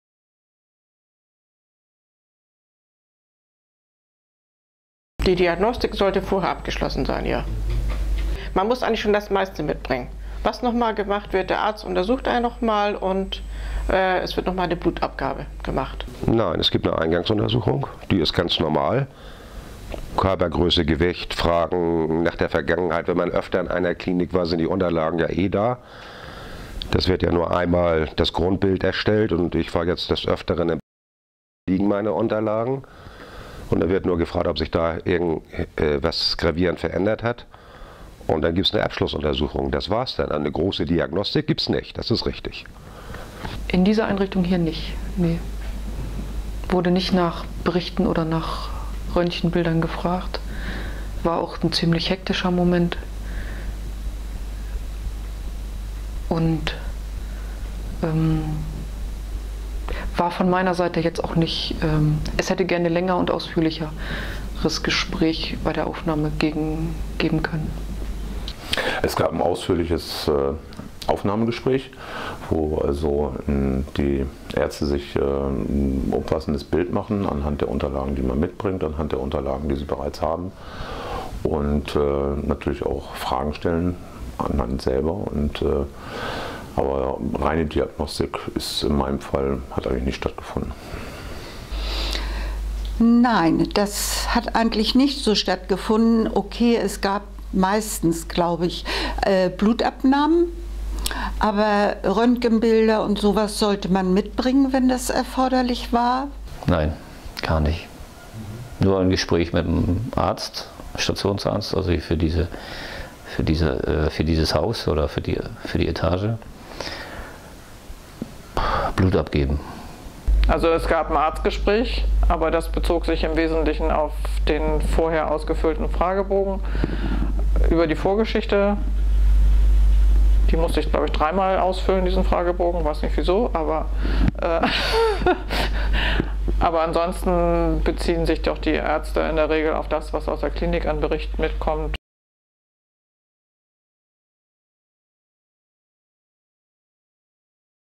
Diagnostik in der Reha
Rehabilitanden sprechen über Diagnostik in der Reha